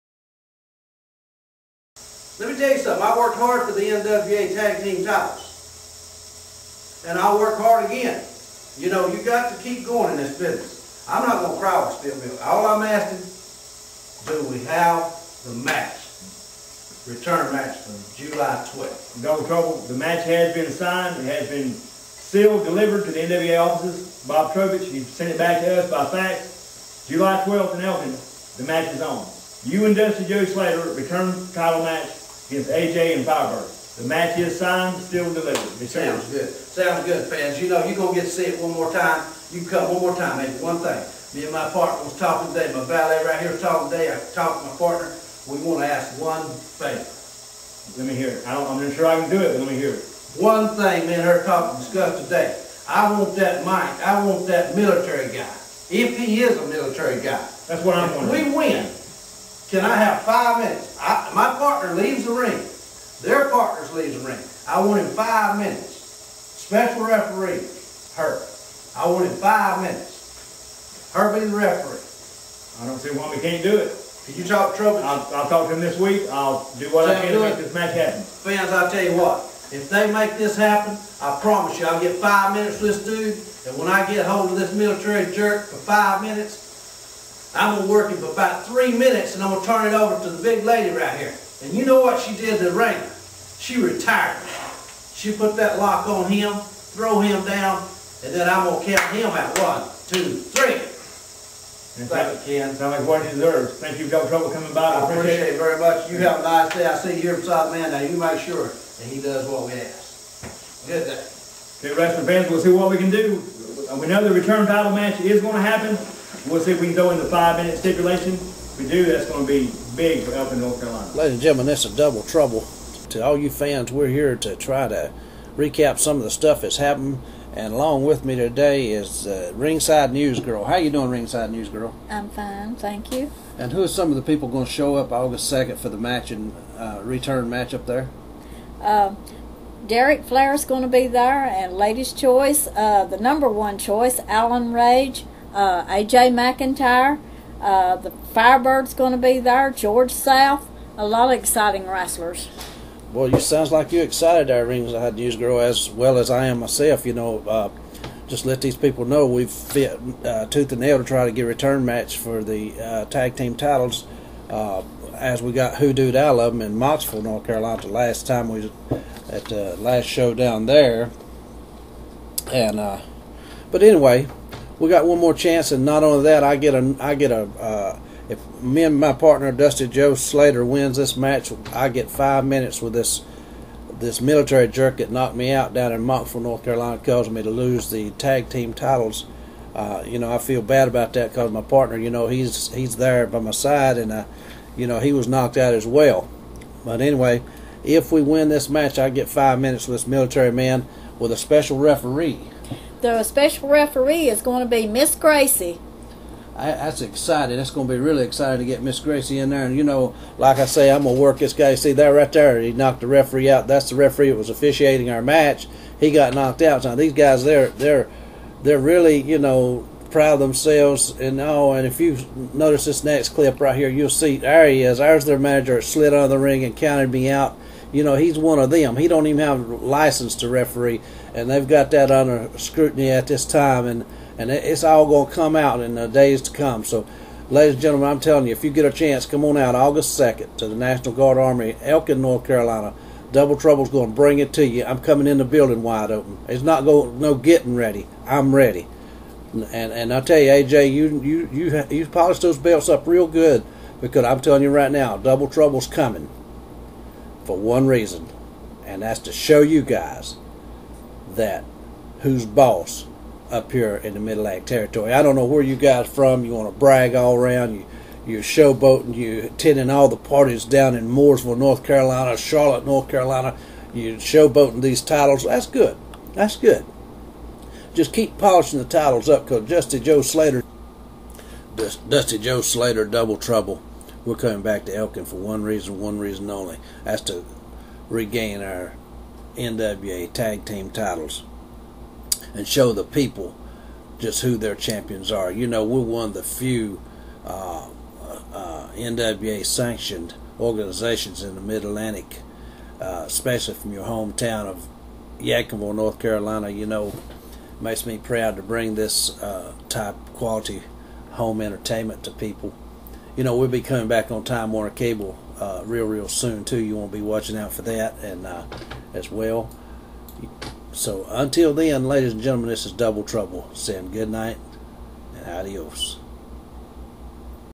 Double Trouble Plead For A Return Match
NWA Carolinas Tag Team Title Match Granted By General Manager Ken Spence August 2, 2008
National Guard Armory
1775 N. Bridge St.
Elkin, N.C. 28621